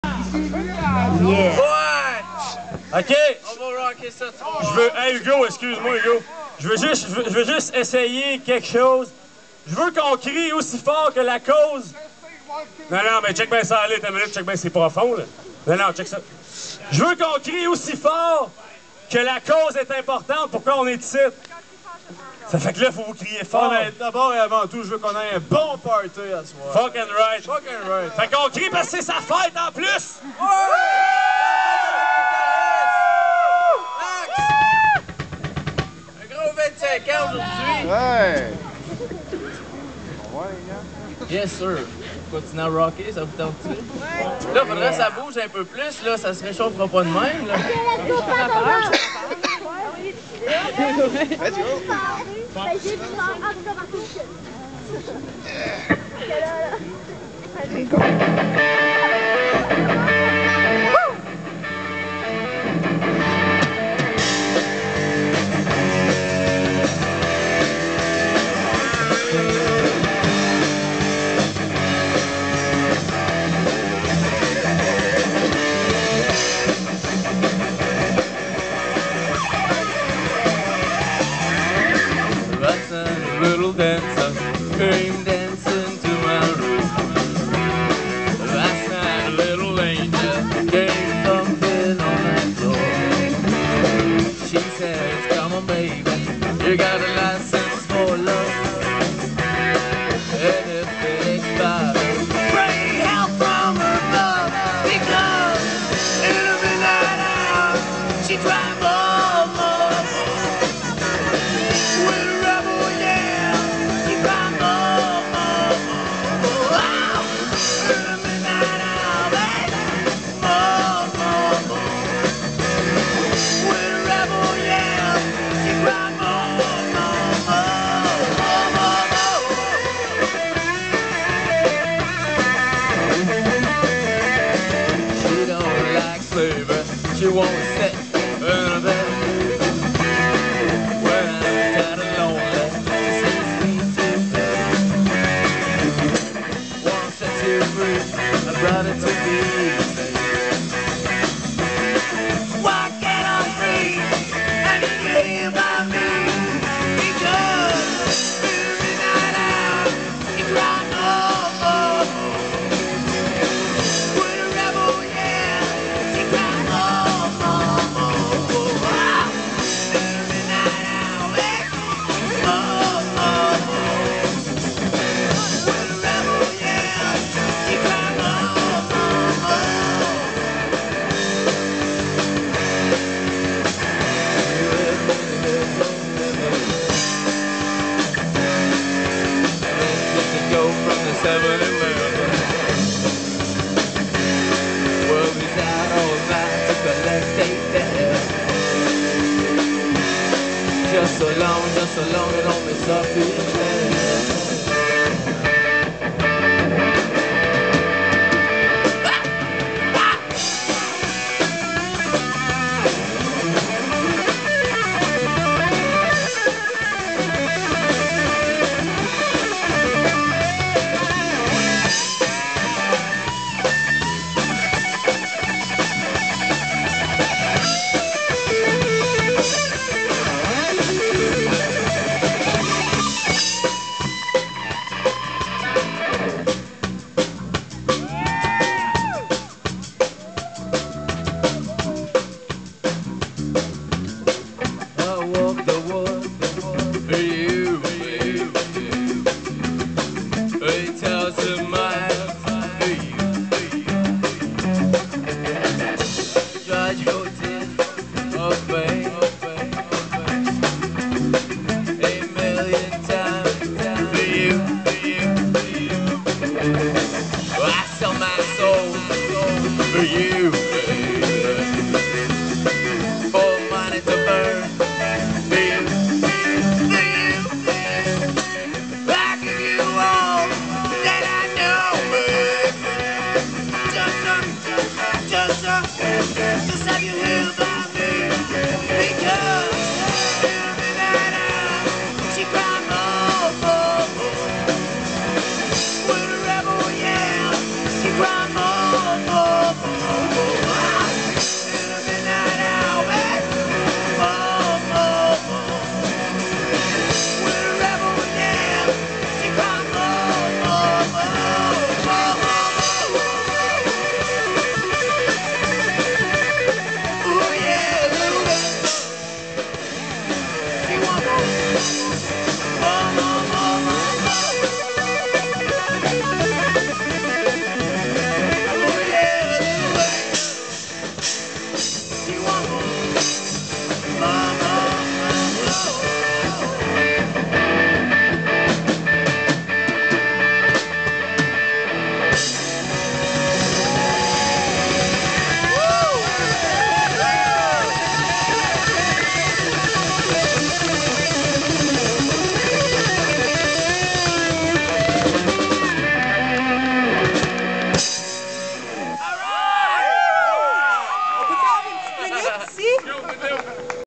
Bridge Illusion live a la Nuit des Sans Abri,
Bridge Illusion avec notre BAssiste Pat, qui à fait un seul gig celuila.

RAWK on bring 's back old old memories !!!